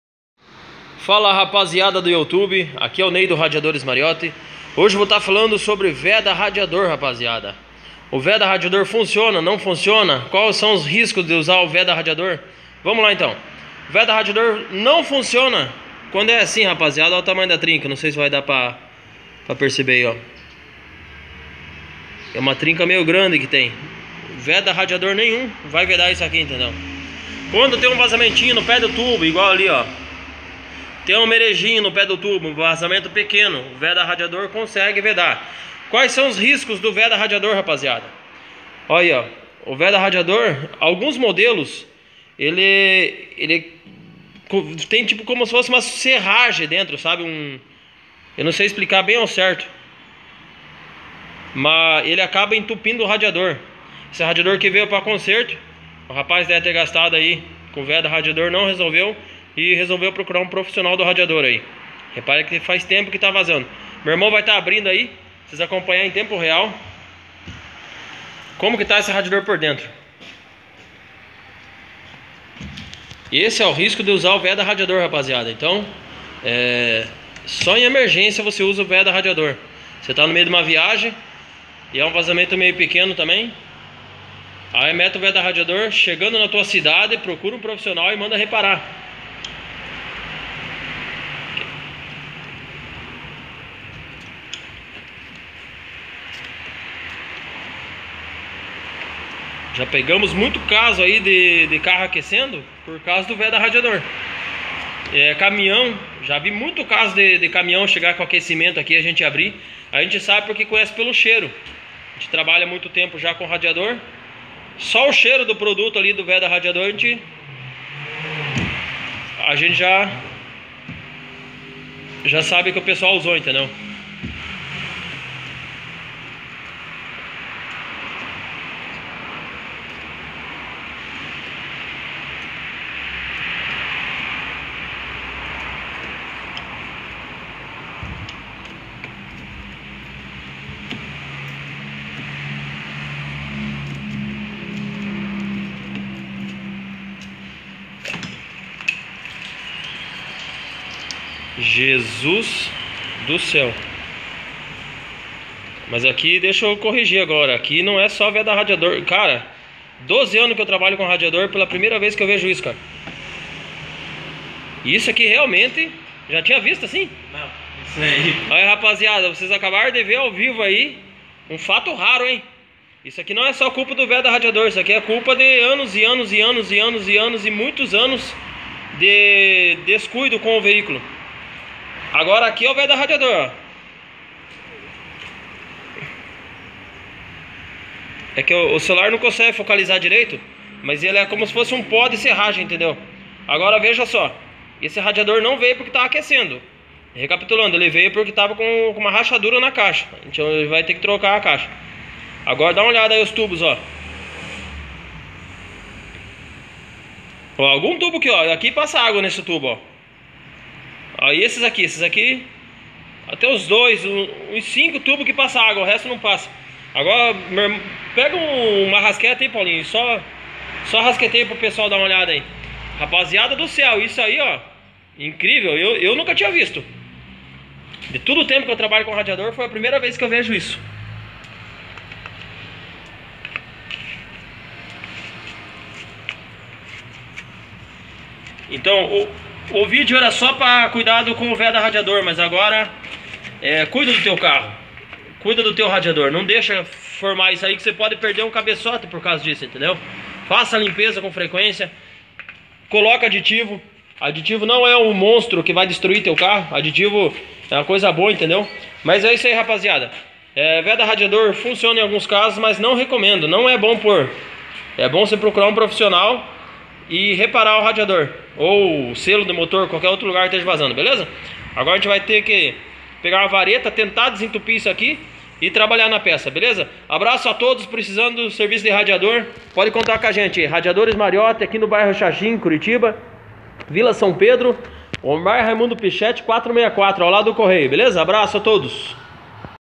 VEDA RADIADOR FUNCIONA??? entenda os riscos do uso em excesso
Fiz esse video para alertar as pessoas, sobre os riscos que "alguns" veda radiador podem causar em seu sistema de arrefecimento... e seu uso em excesso pode comprometer a passagem da água nos tubos gerando aquecimento!!!